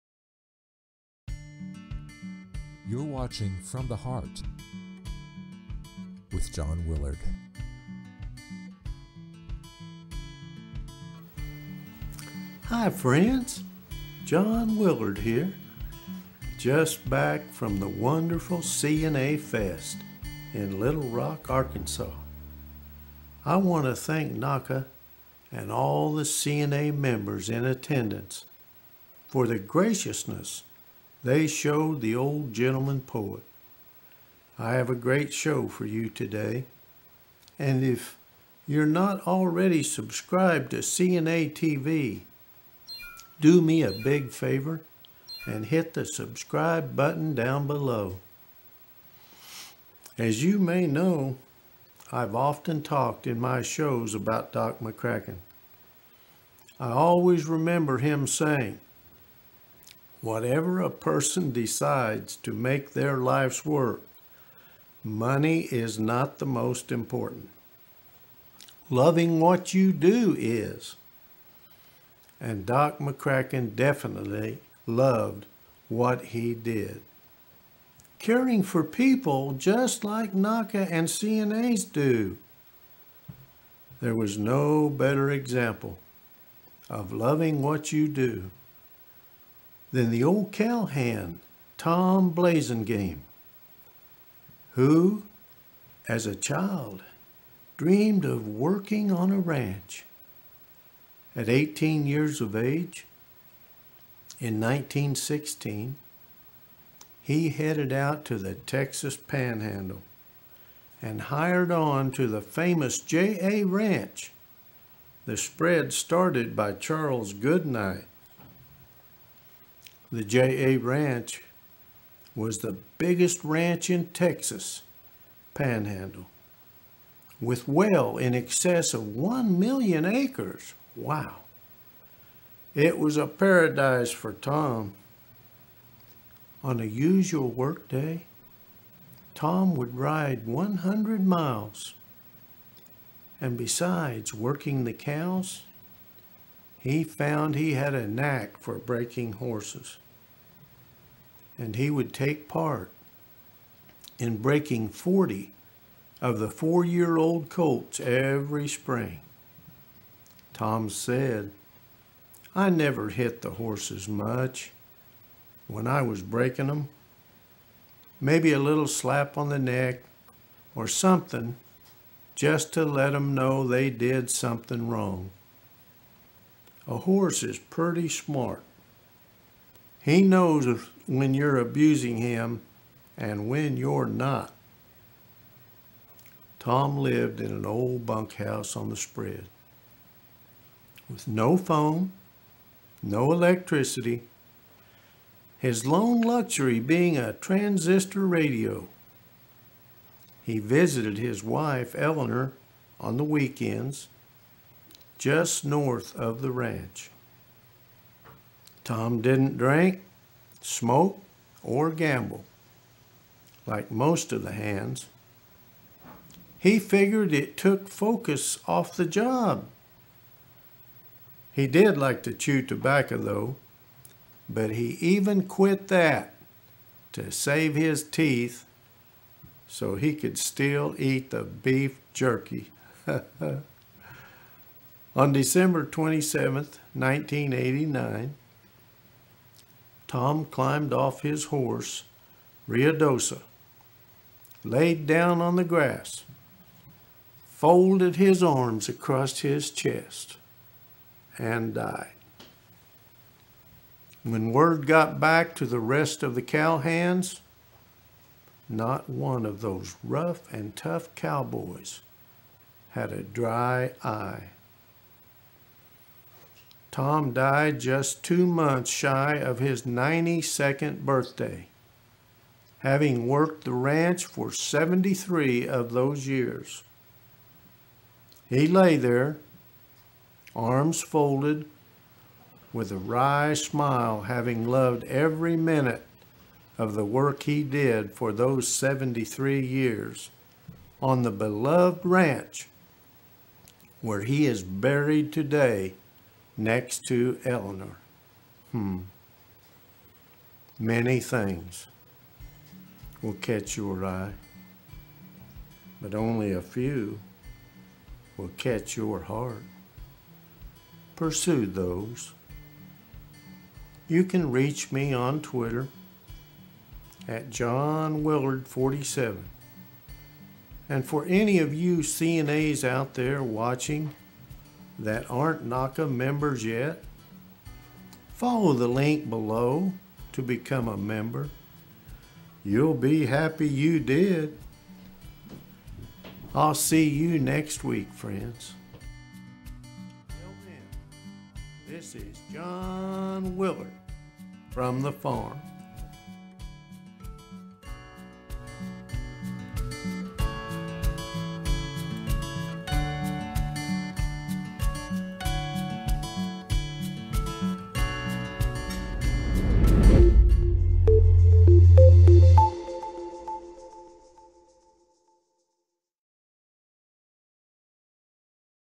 Loving Your Work - From the Heart, with John Willard
Hi friends, John Willard here- just back from the wonderful CNA-FEST In Little Rock, Arkansas. I want to thank NAHCA and all the CNA members in attendance for the graciousness they showed the Old Gentleman Poet!

As you may know I’ve often talked in my shows about Doc McCracken- I always remember him saying- “Whatever a person decides to make their life’s work money is not the most important- loving what you do is!” And Doc McCracken definitely loved what he did- caring for people just like NAHCA and CNA’s!

There was no better example of loving what you do than the old cowhand Tom Blasingame Who as a child dreamed of working on a ranch. At 18 years of age in 1916 he headed out to the Texas Panhandle and hired on to the famous JA Ranch the spread started by Charles Goodnight.

The JA Ranch was the biggest ranch in the Texas Panhandle with well in excess of one million acres- it was a paradise for Tom!

On a usual workday Tom would ride 100 miles and besides working the cows he found he had a knack for breaking horses. And he would take part in breaking 40 four year old colts every spring. Tom said-“I never hit the horses much when I was breakin ‘em, Maybe a little slap on the neck or somethin’ just to let ‘em know they did somethin’ wrong. A horse is purdy smart. He knows when your abusing him and when you’re not.”

Tom lived in an old bunk House on the spread with no phone or electricity, his lone luxury being a transistor radio. He visited his wife Eleanor on the weekends just north of the ranch. Tom didn’t drink, smoke or gamble like most of the hands, he figured it took focus off of the job- he did like to chew tobacco though but even quit that to save his teeth so he could still eat beef jerky.

On December 27, 1989, Tom climbed off his horse Ruidosa, laid down on the grass, folded his arms across his chest and died. When word got back to the rest of the cowhands not one of those rough and tough Cowboys had a dry eye! Tom died just two months shy of his 92nd birthday having worked the ranch for 73 of those years! He lay there, arms folded with a wry smile having loved every minute of the work he did for those 73 years on the beloved ranch where he is buried today next to Eleanor.

Many things will catch your eye- but only a few will catch your heart- pursue those.

You can reach me on Twitter @JohnWillard47 and for any of you CNA’s out there watching that aren’t NAHCA members yet follow the link below to become a member- you’ll be happy you did.

This is John Willard, from the Farm!